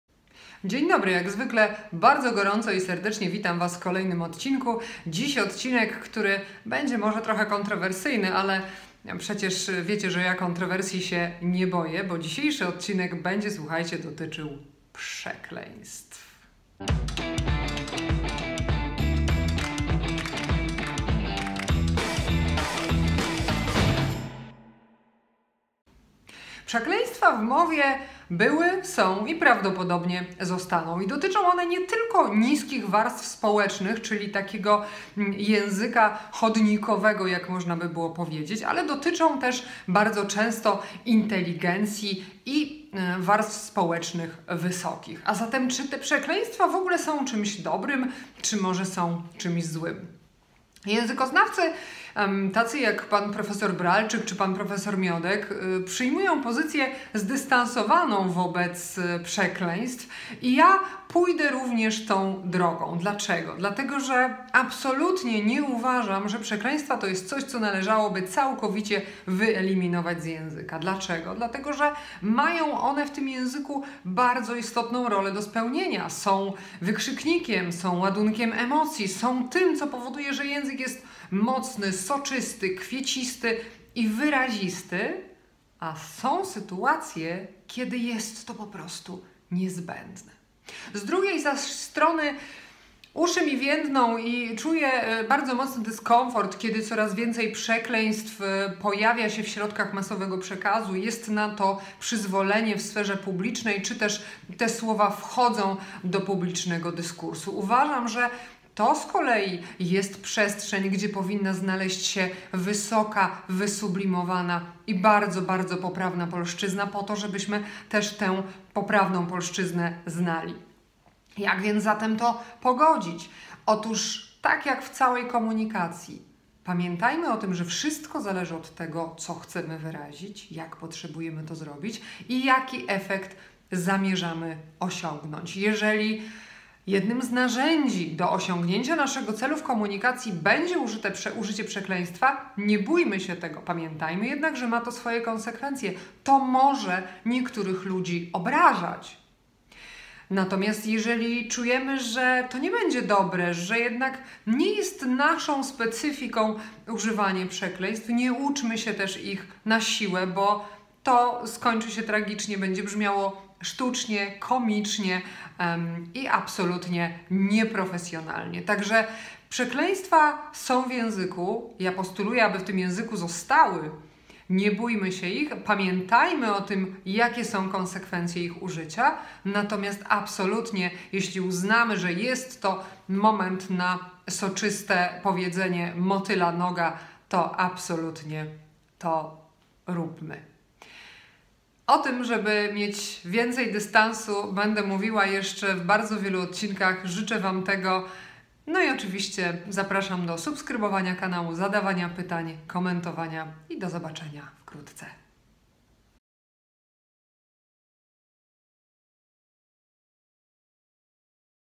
Czy warto przeklinać? #010
Każdemu z nas zdarza się przekląć. Przekleństwa były, są i na zawsze pozostaną w języku. Nie są niczym złym, jeśli tylko wiemy kiedy i jak ich używać. Musimy także zdawać sobie sprawę z ich konsekwencji. Niektórych przeklinanie może bawić, innych - obrażać. Trzeba więc wiedzieć, jak i do kogo mówić.